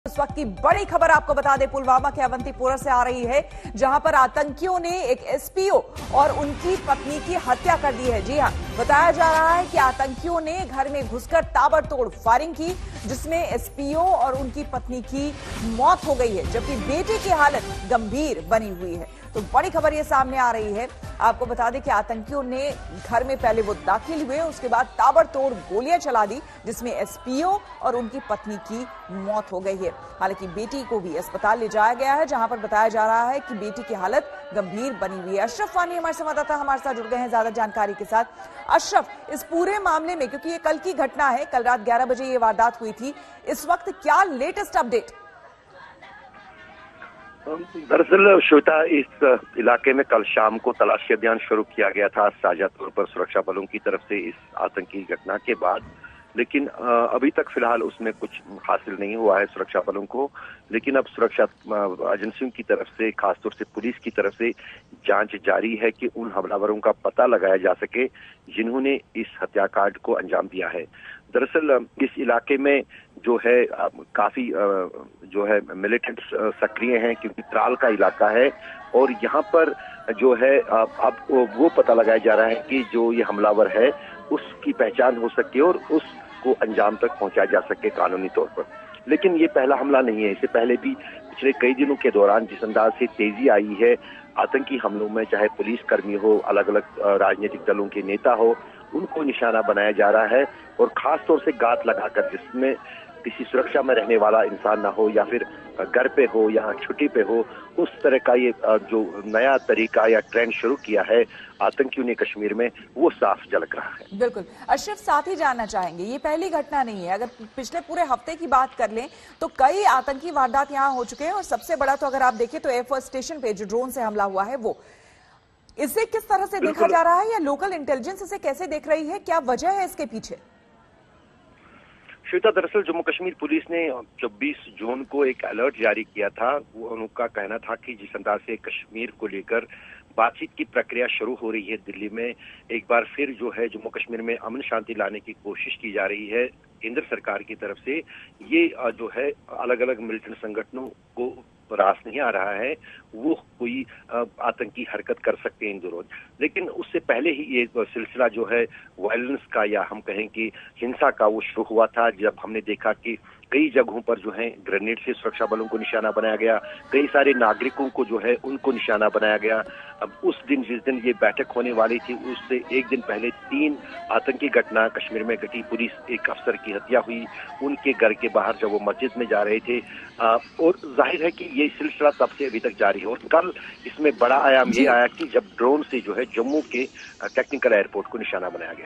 Breaking News : Pulwama आतंकी हमले में SPO और पत्नी की हत्या, बेटी की हालत गंभीर
जम्‍मू में ड्रोन हमले के 24 घंटे के भीतर ही आतंकियों ने कश्‍मीर के पुलवामा में पूर्व स्‍पेशल पुलिस ऑफिसर (एसपीओ) फैयाज अहमद की गोली मारकर हत्‍या कर दी. पुलवामा में अवंतीपुरा के हरिपरिगाम गांव में आतंकी जम्‍मू एवं कश्‍मीर पुलिस में एसपीओ रहे फैयाज अहमद के घर में घुस गए और अंधाधुंध गोलियां चलाईं.